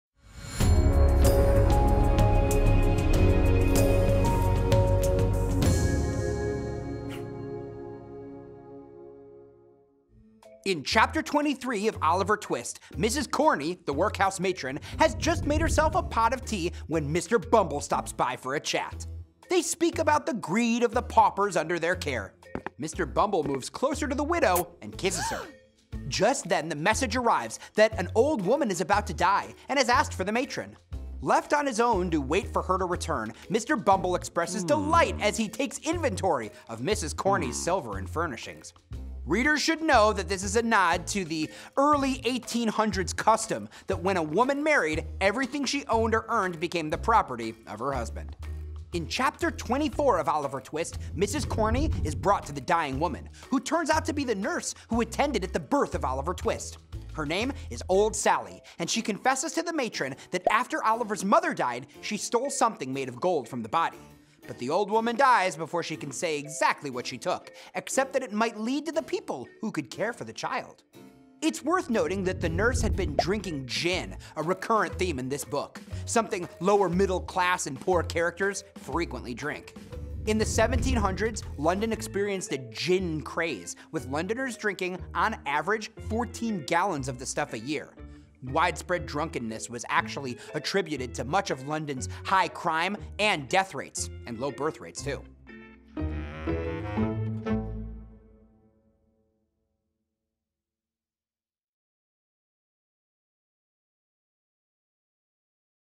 Oliver Twist by Charles Dickens | Chapters 23–24
Summarize videos instantly with our Course Assistant plugin, and enjoy AI-generated quizzes Charles Dickens’s Oliver Twist explained with chapter summaries in just a few minutes!

Course Hero Literature Instructor Russell Jaffe provides an in-depth summary and analysis of Chapters 23–24 of Charles Dickens's novel Oliver Twist.

Download the free study guide and infographic for Oliver Twist

Charles Dickens’s Oliver Twist is the story of a young orphan boy in early Victorian England, when the poor were treated like criminals.

After escaping the confines of a workhouse, orphan Oliver is almost immediately ensnared by the devious Fagin. Fagin seeks to employ him and other children as pickpockets. Oliver is aided by several of his well-intentioned victims and ultimately escapes a life of crime when he comes into an inheritance.

His path to fulfillment is rife with topical impediments: the Victorian era was one of class strife and the exploitation of children was far from rare. Nearly thwarted by his enemies, he exhibits the spritely intelligence and resourcefulness typical of Dickens's beloved protagonists and perseveres against the odds.

Oliver Twist was first published in 1837-39. Charles Dickens, a prolific Victorian author, loved a rags to riches story. Perhaps most emblematic of this fixation is the titular hero of Oliver Twist. Rife with quirky villains, saintly saviors, and ingenious urchins, Oliver Twist embodies the timelessly enchanting sagas for which the premier novelist of the Victorian age is known.

The novel Oliver Twist contains many powerful themes, including poverty, as society punishes rather than helps the poor. Virtue versus evil is another important theme, as Oliver’s virtuous heart proves incorruptible and the evildoers are punished. Criminality is also explored, as the poor are often forced into a life of crime. Important symbols include obesity, the countryside, and darkness.